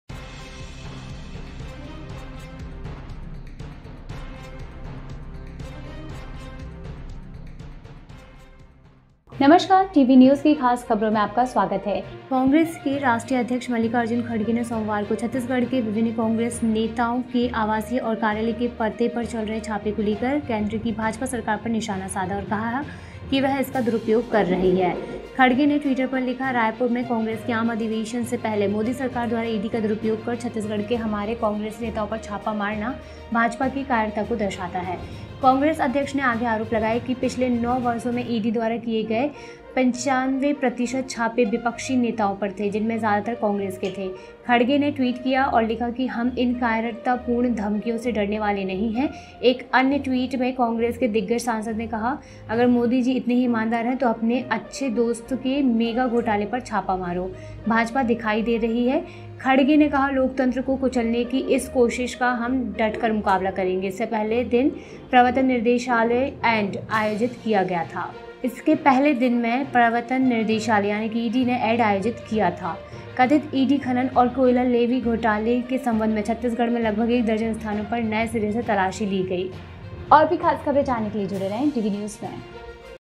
Kharge: Condemned ED raids on leaders | नेताओं पर ईडी के छापे की निंदा की | Congress | Chhattisgarh
कांग्रेस के राष्ट्रीय अध्यक्ष मल्लिकार्जुन खड़गे ने सोमवार को छत्तीसगढ़ के विभिन्न कांग्रेस नेताओं के आवासीय और कार्यालय के पते पर चल रहे छापे को लेकर केंद्र की भाजपा सरकार पर निशाना साधा और कहा कि वह इसका दुरुपयोग कर रही है। राज्य में सत्तारूढ़ पार्टी के खिलाफ केंद्रीय एजेंसी।

Congress national president Mallikarjun Kharge on Monday hit out at the BJP government at the Center over the ongoing raids at the residential and office addresses of various Congress leaders in Chhattisgarh, saying it was misusing it. Central agency against the ruling party in the state.